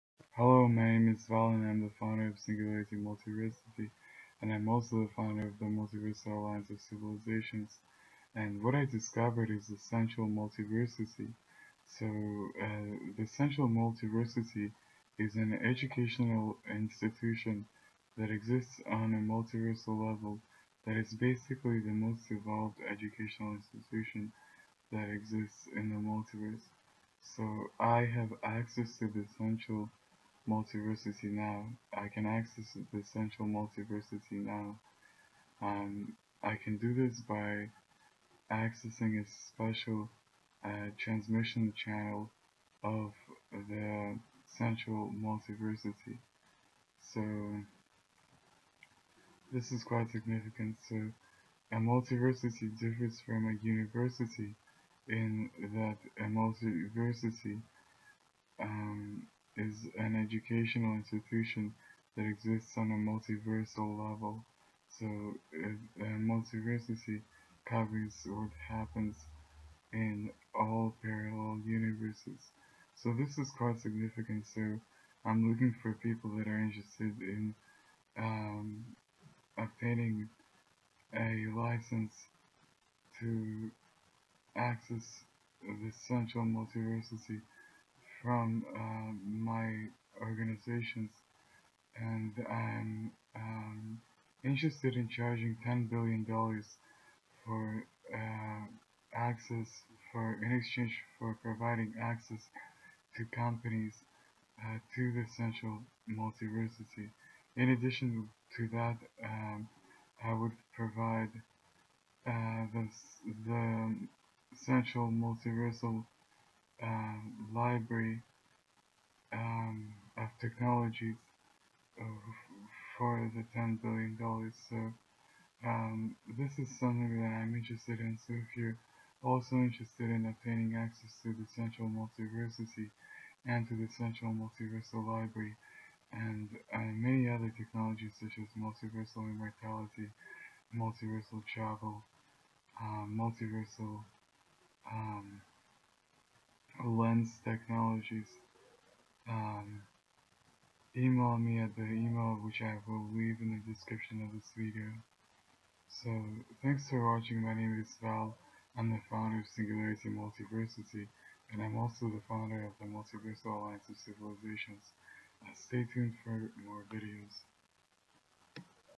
I discovered and can access the Central Multiversity
Hello my name is Val and I am the founder of Singularity Multiversity and the Multiversal Alliance of Civilizations I discovered and can access the Central Multiversity. The Central Multiversity is the most evolved educational institution in the multiverse and I currently have access to an information transmission channel of the Central Multiversity and I am looking for companies that are interested in accessing the Central Multiversity and many other technologies such as Multiversal Immortality and Multiversal Travel for a price of $10 billion dollars then email me at